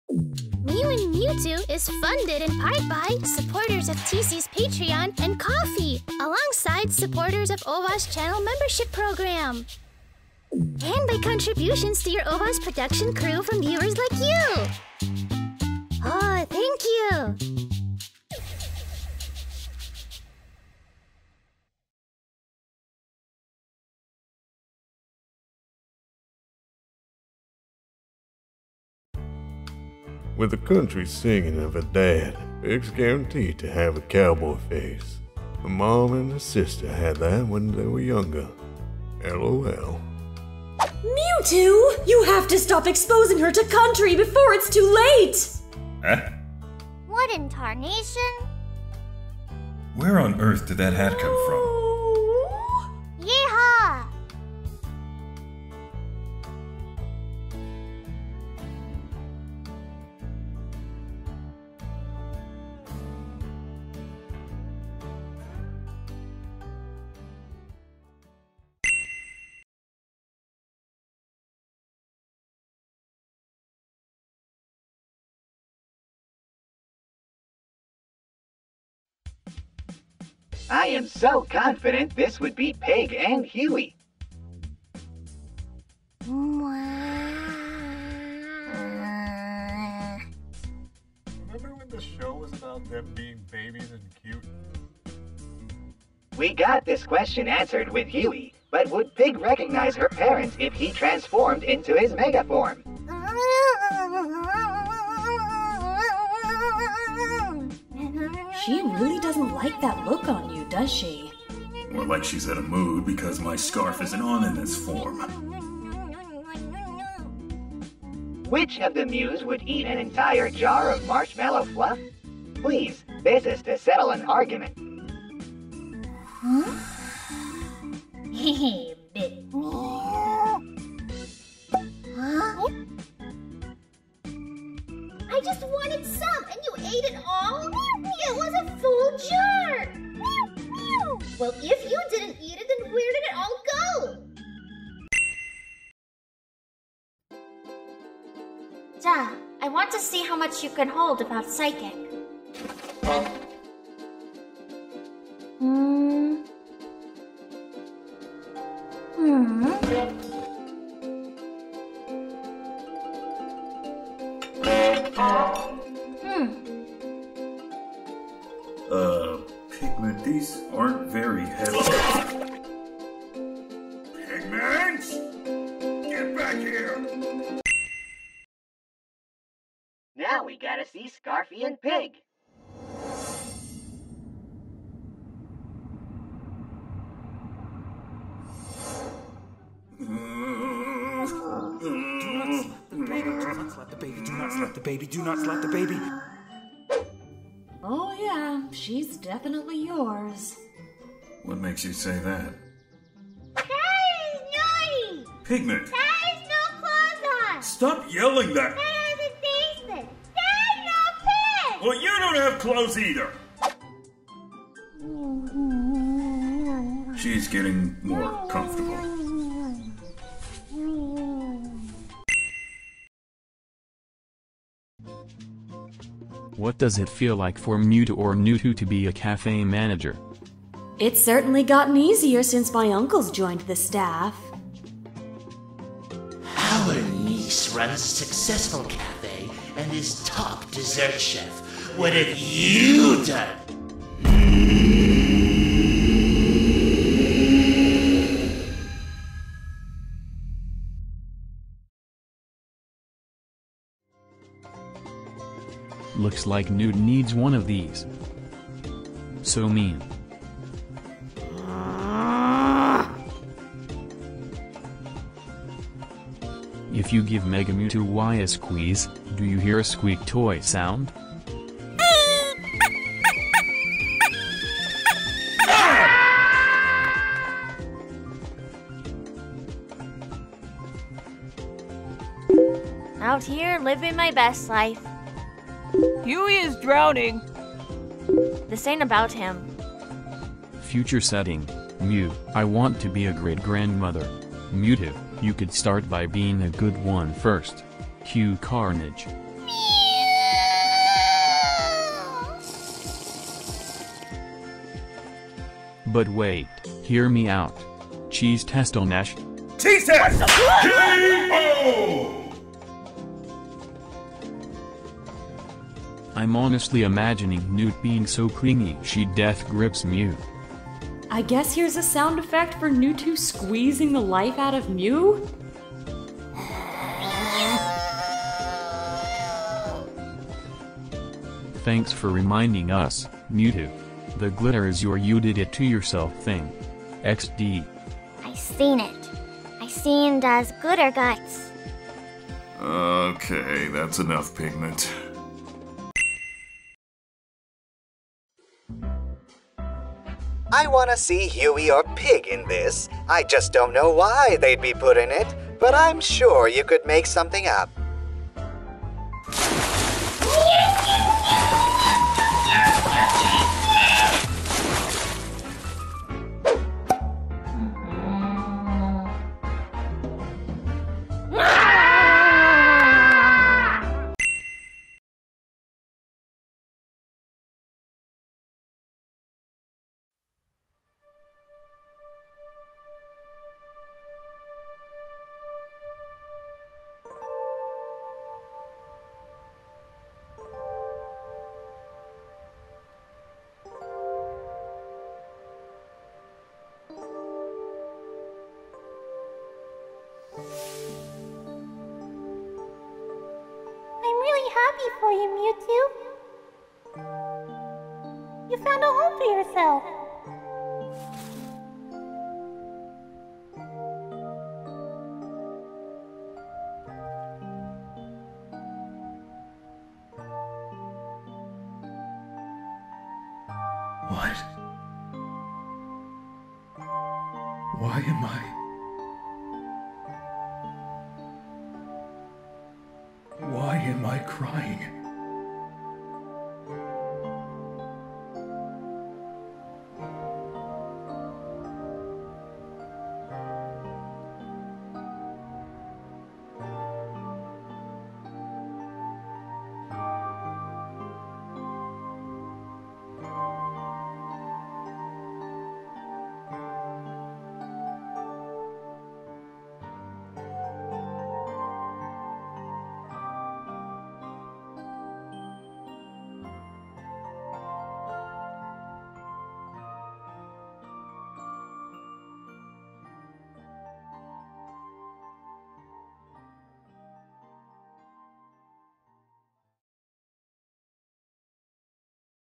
Mew & Mewtwo by TC-96 [Comic Drama Part #58]
► A fun little compilation to welcome our new Pig VA, Leafuchii!

CHECK OUT TC-96 for more Mew & Mewtwo as well as some other content! Be sure to send your support their way! 💜

Thank you as always to Kn, Sushi, and James for voicework! Welcome Leafuchii! Also shout-out to [the Pokemon we caught] for creating the jingle for the Mew & Mewtwo PBS intro! ❤️

CAST
WolfBlade93 as Mewtwo

SOUND DESIGN by: James Dijit
EDITED by: PCShnun